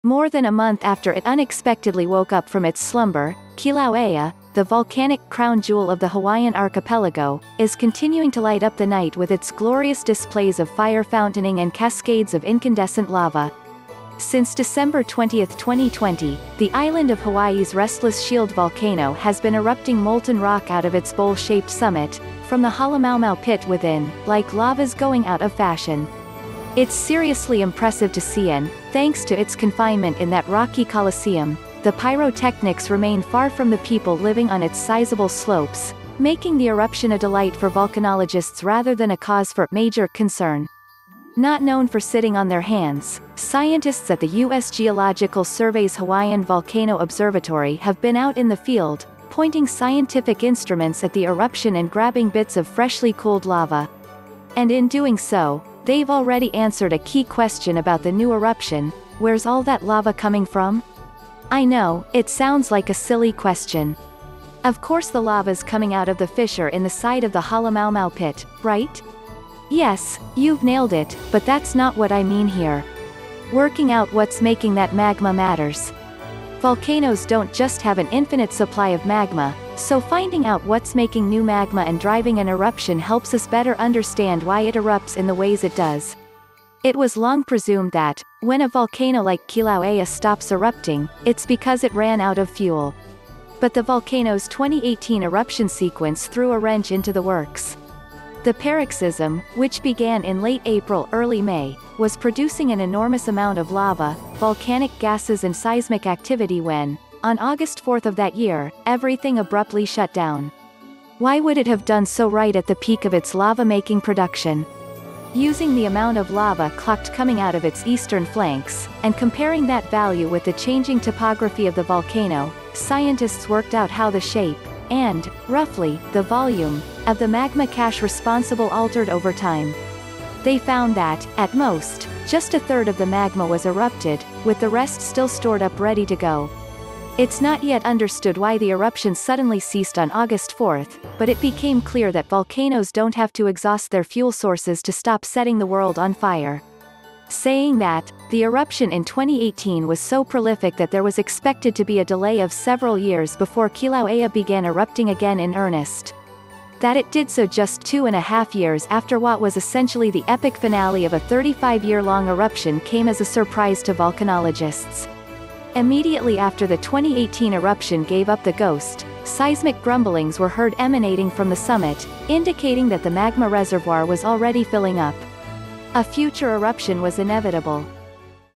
Hawaii’s Kīlauea Volcano Looks Like It’s Clearing Its Throat
More than a month after it unexpectedly woke up from its slumber, Kīlauea, the volcanic crown jewel of the Hawaiian archipelago, is continuing to light up the night with its glorious displays of fire fountaining and cascades of incandescent lava. Since December 20th, 2020, the Island of Hawai‘i’s restless shield volcano has been erupting molten rock out of its bowl-shaped summit, from the Halema‘uma‘u pit within, like lava’s going out of fashion. It’s seriously impressive to see and, thanks to its confinement in that rocky coliseum, the pyrotechnics remain far from the people living on its sizeable slopes — making the eruption a delight for volcanologists rather than a cause for (major) concern. Not known for sitting on their hands, scientists at the U. S. Geological Survey’s Hawaiian Volcano Observatory have been out in the field, pointing scientific instruments at the eruption and grabbing bits of freshly cooled lava. And in doing so, they’ve already answered a key question about the new eruption: where’s all that lava coming from? I know, it sounds like a silly question. Of course the lava’s coming out of the fissure in the side of the Halema‘uma‘u pit, right? Yes, you’ve nailed it, but that’s not what I mean here. Working out what’s making that magma matters. Volcanoes don’t just have an infinite supply of magma, so finding out what’s making new magma and driving an eruption helps us better understand why it erupts in the ways it does. It was long presumed that, when a volcano like Kīlauea stops erupting, it’s because it ran out of fuel. But the volcano’s 2018 eruption sequence threw a wrench into the works. The paroxysm, which began in late-April/early-May, was producing an enormous amount of lava, volcanic gases and seismic activity when, on August 4th of that year, everything abruptly shut down. Why would it have done so right at the peak of its lava-making production? Using the amount of lava clocked coming out of its eastern flanks, and comparing that value with the changing topography of the volcano, scientists worked out how the shape – and, roughly, the volume – of the magma cache responsible altered over time. They found that, at most, just a third of the magma was erupted, with the rest still stored up ready to go. It’s not yet understood why the eruption suddenly ceased on August 4th, but it became clear that volcanoes don’t have to exhaust their fuel sources to stop setting the world on fire. Saying that, the eruption in 2018 was so prolific that there was expected to be a delay of several years before Kīlauea began erupting again in earnest. That it did so just two-and-a-half years after what was essentially the epic finale of a 35-year-long eruption came as a surprise to volcanologists. Immediately after the 2018 eruption gave up the ghost, seismic grumblings were heard emanating from the summit, indicating that the magma reservoir was already filling up. A future eruption was inevitable.